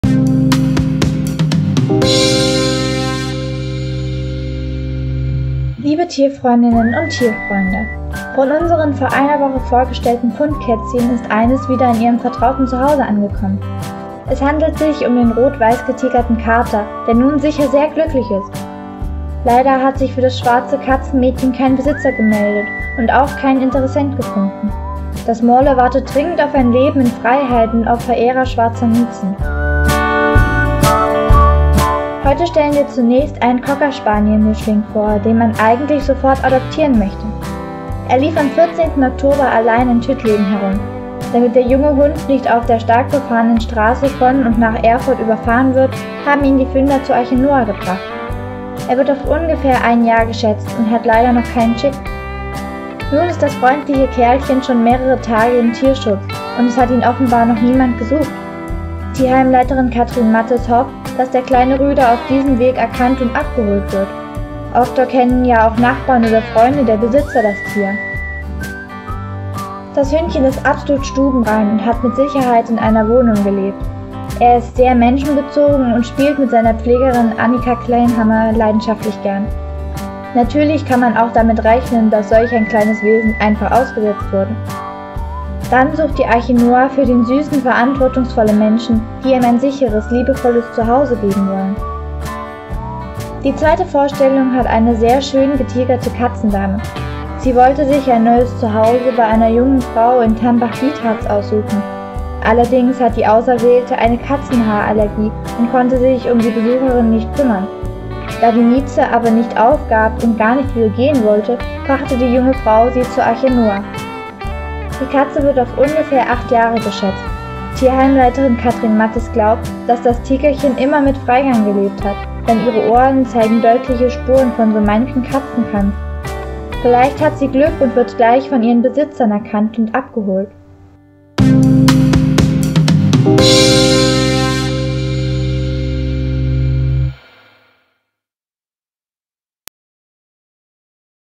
Junger Stromer aus Tüttleben
Heute stellen wir einen Fundhund aus Tüttleben und außerdem eine Fundkatze vor, die aus Tambach-Dietharz stammt. Mehr Informationen im Tierheim Gotha unter Tel. 03621/ 755 425.